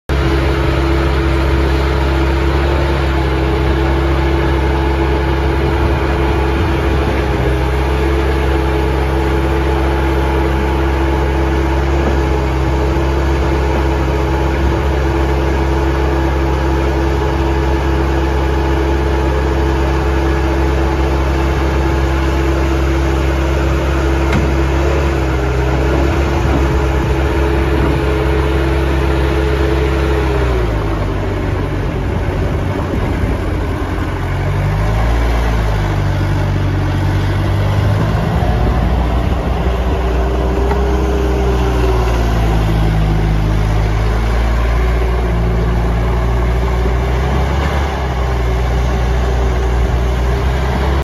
2005 BOBCAT T250 For Sale
OROPS, 2216 Hours, Hand & Foot Controls, AUX Hydraulics, 81 HP Kubota Diesel, 86” GP Bucket, SSL Quick Attach, 18” Rubber Tracks, 2500 LBS Lift Capacity, 9347 LBS Operating Weight, As-Is, Head Gasket Issue, Located at McGrew Equipment Company (Seven Valleys PA)